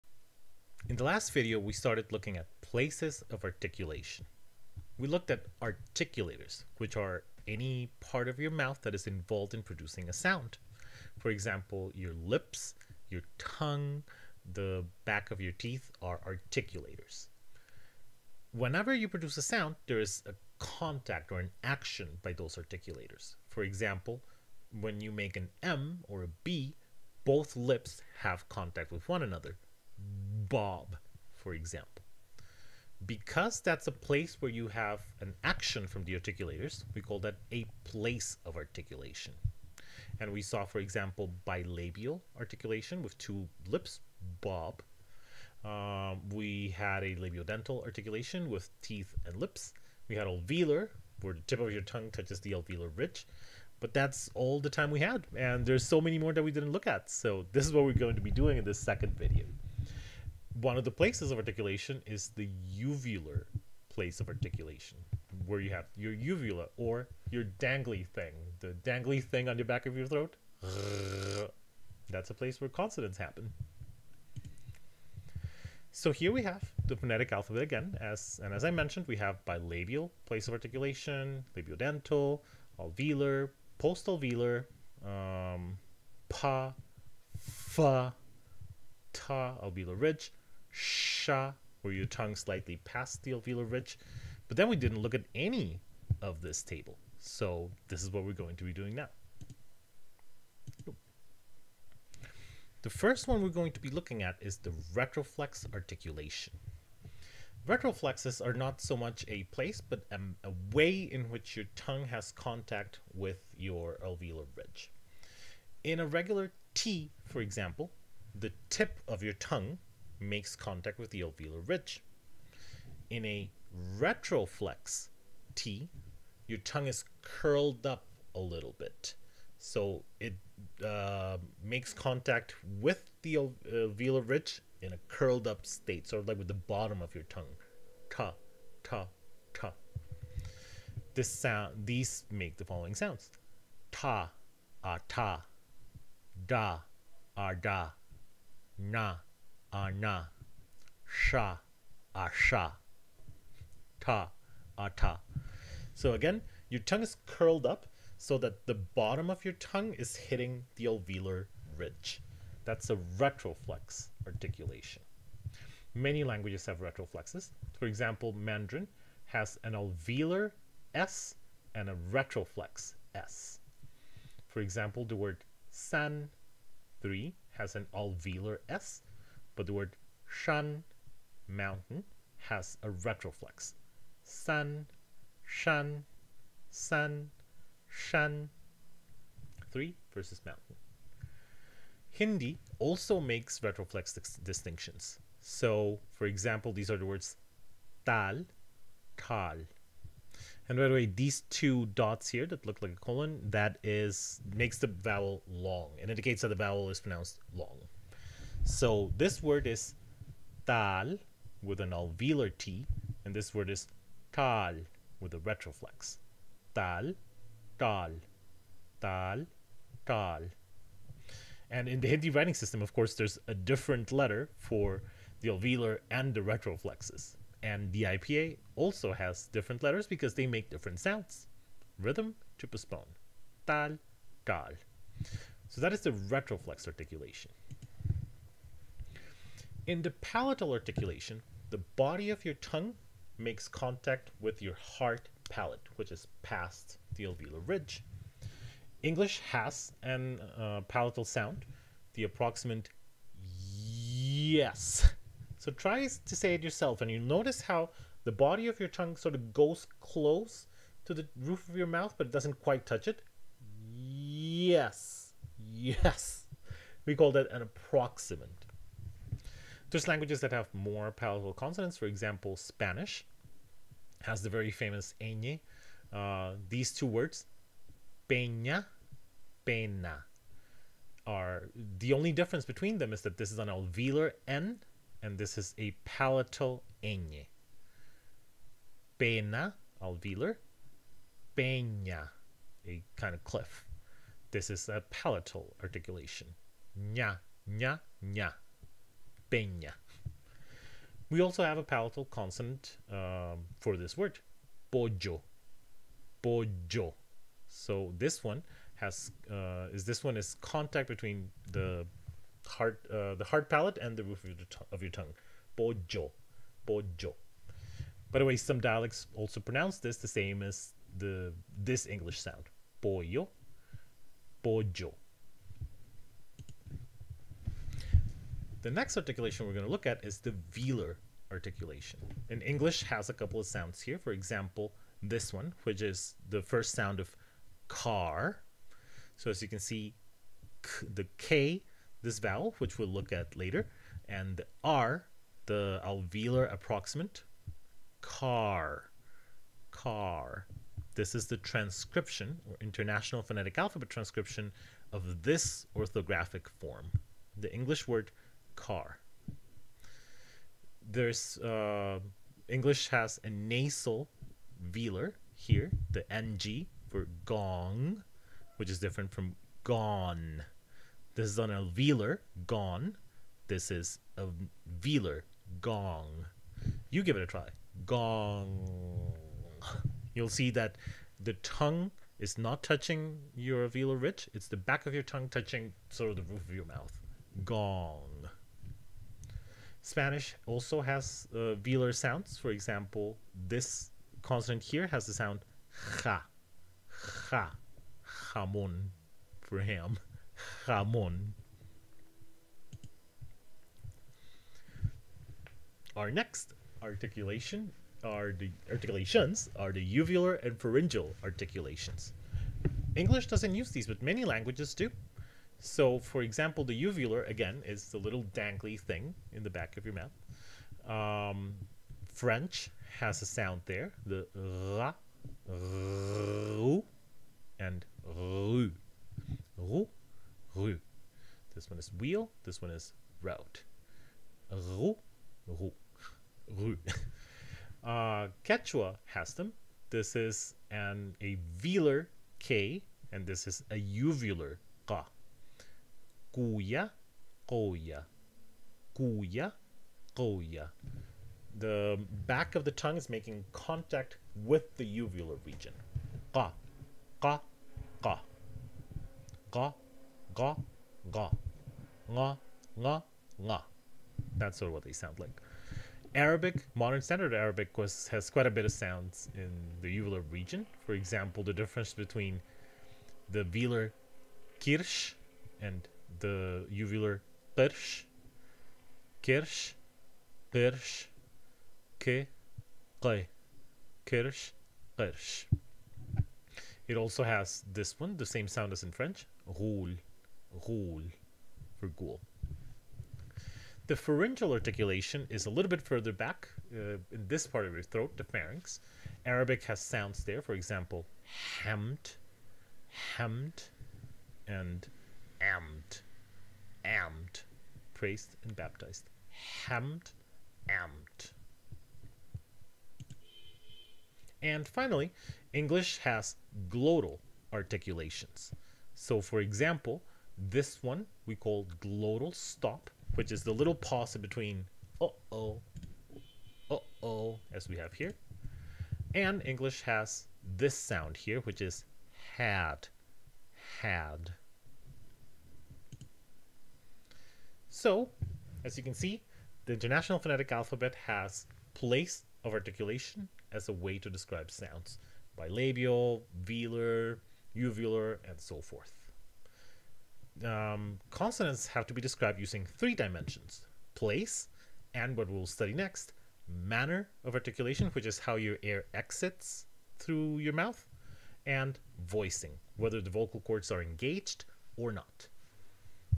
Consonants: Place of Articulation 2 (IntroLing 2020F.W02.03)
Dartmouth College LING001 Fall 2020.
Week 02, Video 03: Consonants: Place of Articulation 2

Contents
00:00 Place of articulation review
01:41 Retroflex
03:59 Palatal
05:27 Velar
06:23 Uvular and pharyngeal
08:33 Glottal
08:57 Summary